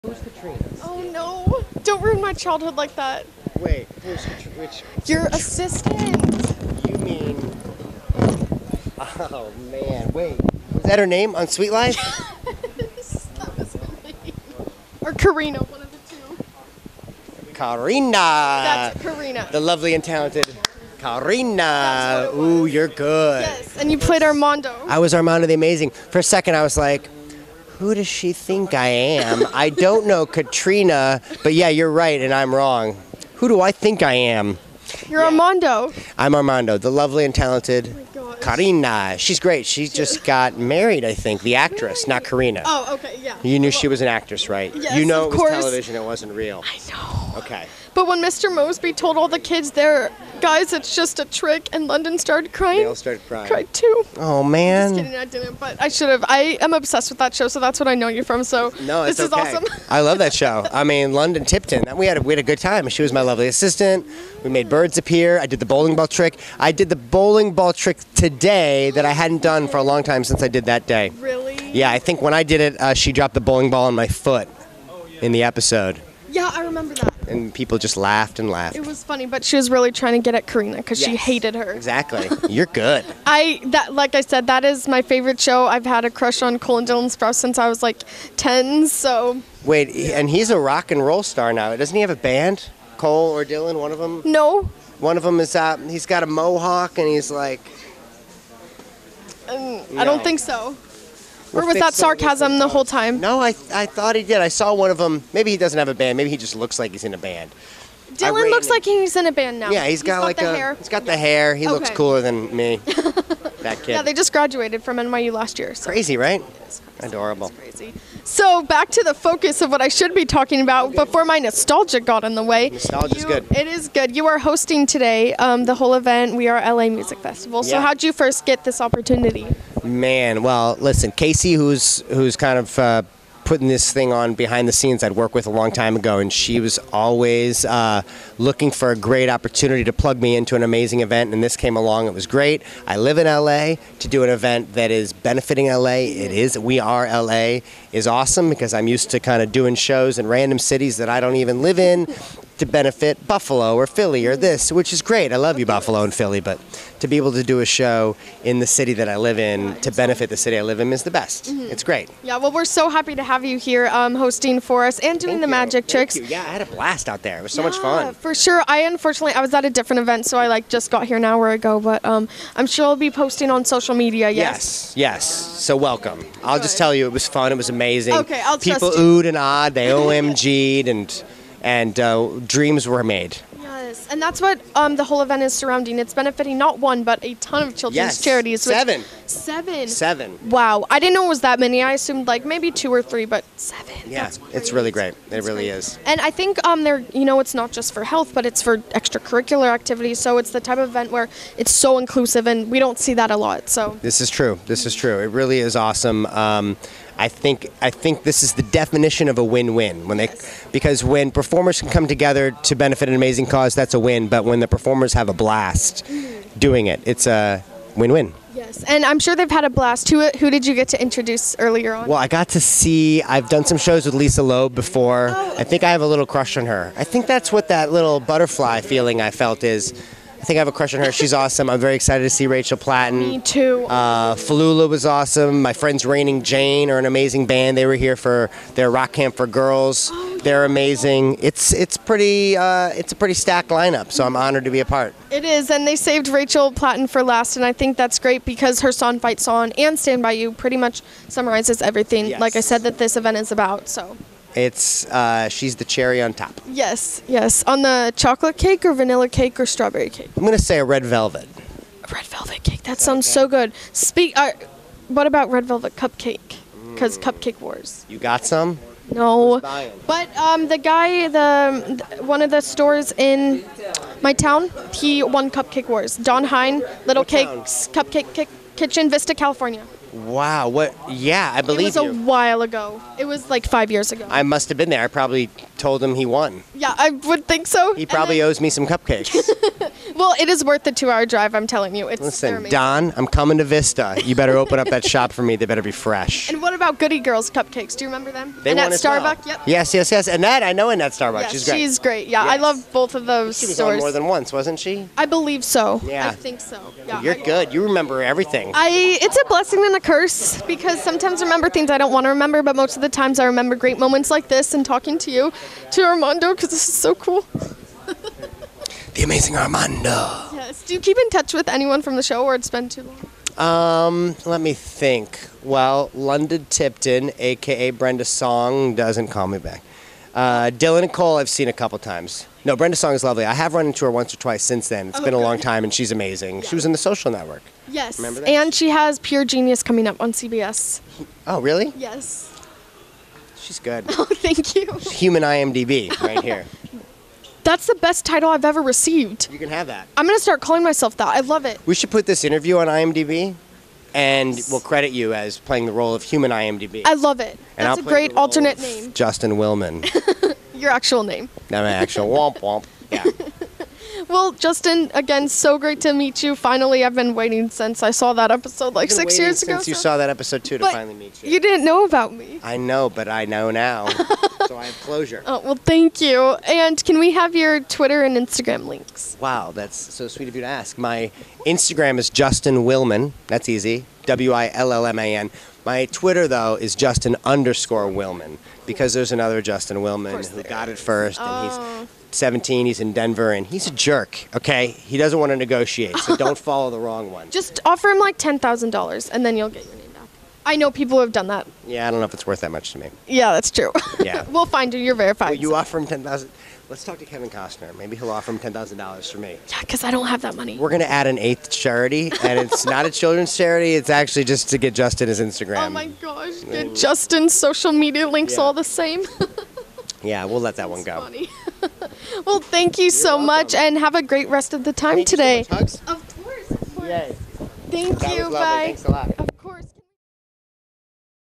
Justin Willman Interview at We Are LA Music Festival
I spoke to Justin Willman at the We Are LA Music Festival, an annual event benefitting 7 different children charities throughout LA! Keep up with their vision and missions